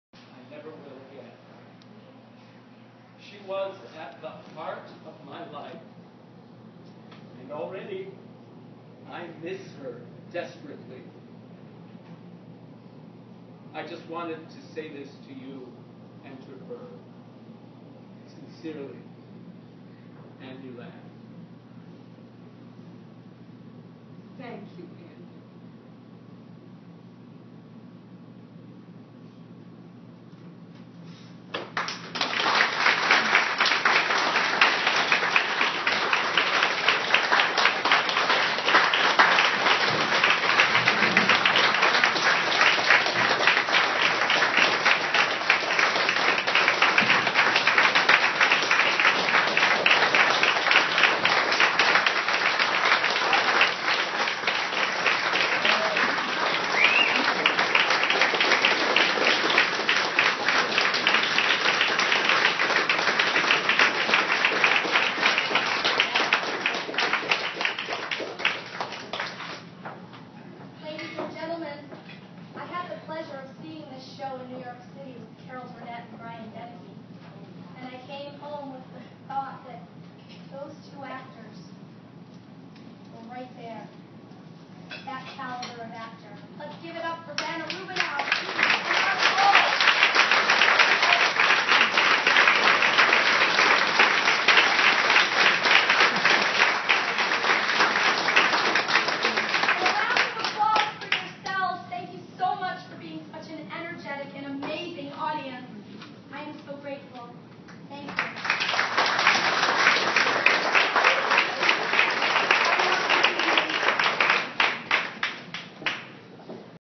"Love Letters" Curtain Call - Bistro 197
Mark Cole & Banna Rubinow take their bows at our sold out Dinner Theatre Experience at Bistro 197 in Oswego, NY! February 10, 2015